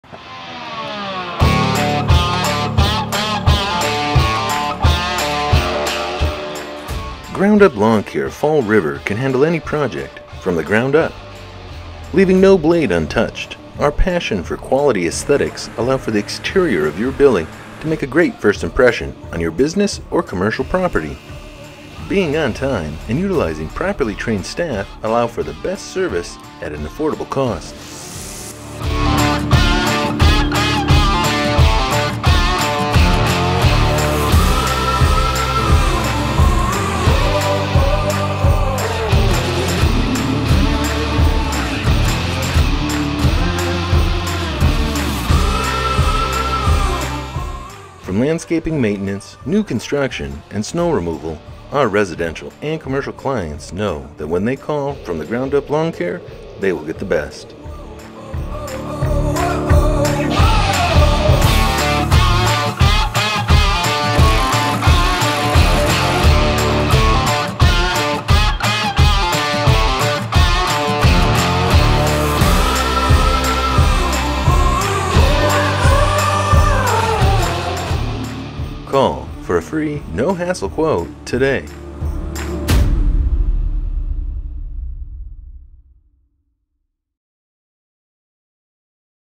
From the Ground Up Lawncare, LLC a Columbus Wisconsin Area Chamber Member 2020
From The Ground Up LawnCare, LLC was founded with one goal in mind - to provide a full range superior lawn maintenance services, including landscaping and lawn maintenance, at affordable rates to the residents of Columbus and surrounding communities.

We are dedicated to building long term relationships with our clients by providing the highest standard of quality on each and every project we do. Our reputation for customer satisfaction in the Columbus area is what has made From The Ground Up LawnCare, LLC a household name in the industry.

At From The Ground Up LawnCare, LLC , we understand that the outdoor aesthetic of your home or business is as important as your interior. From landscape maintenance to new construction, our residential and commercial clients in Columbus know that when they call From The Ground Up LawnCare, LLC they will get the best.